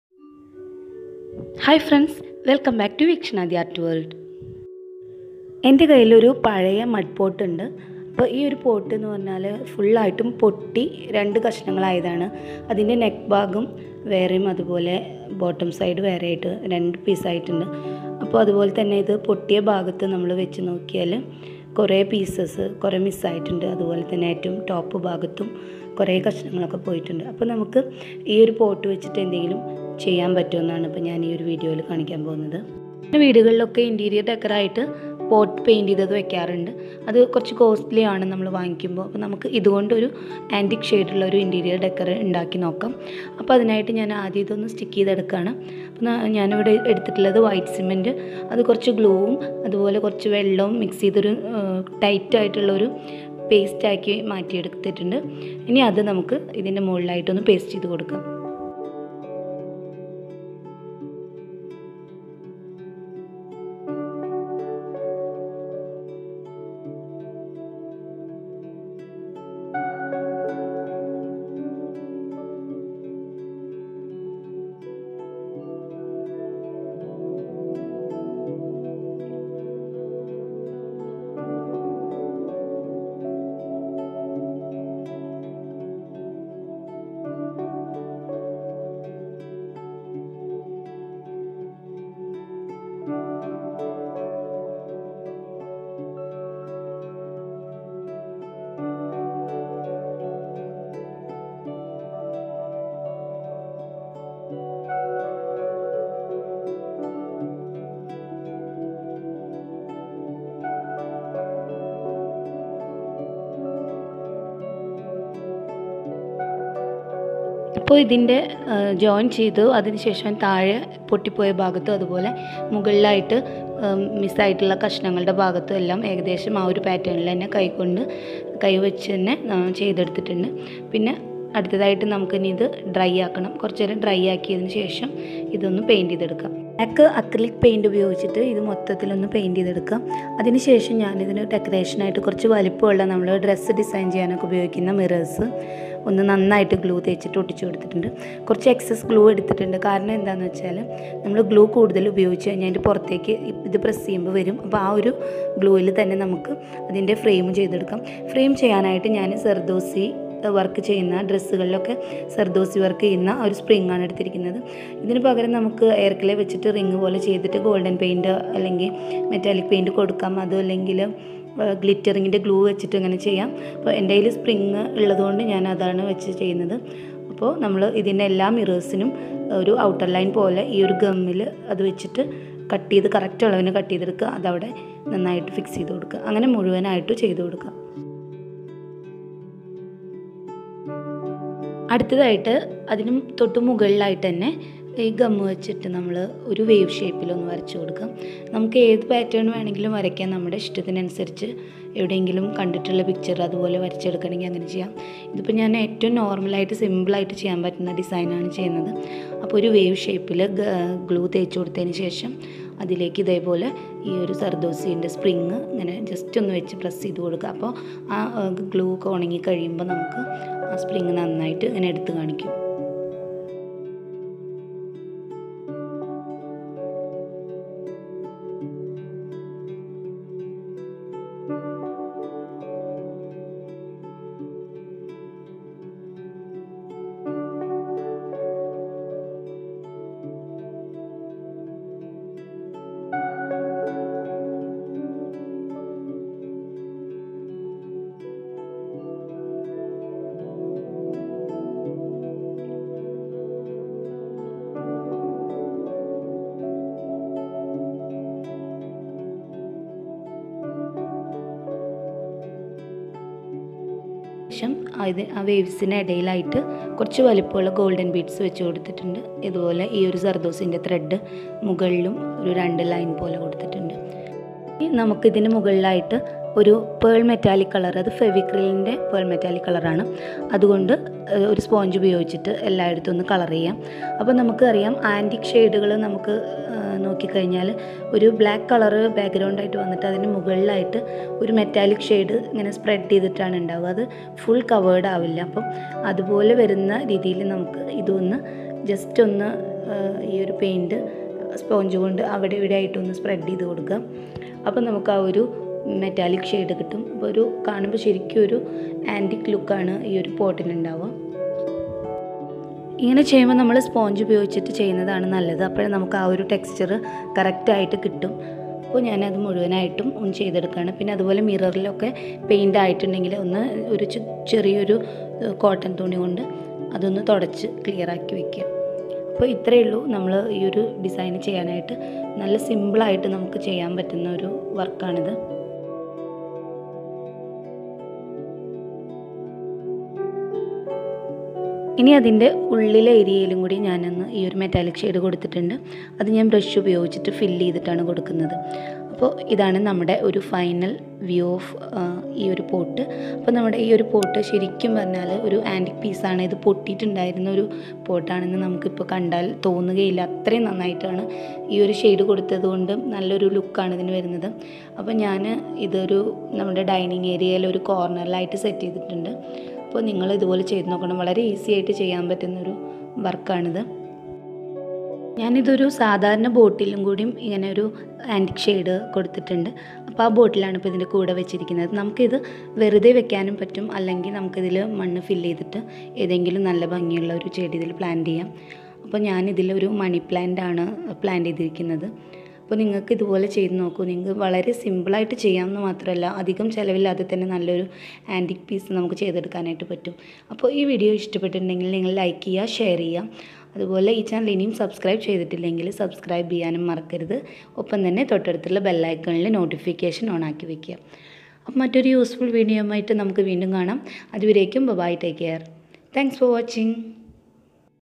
Antique metal art on broken mud pot/reuse/pot painting/easy method
This video how to reuse broken and convert to an amazing antique piece Antique metal design on broken mud pot/reuse/pot painting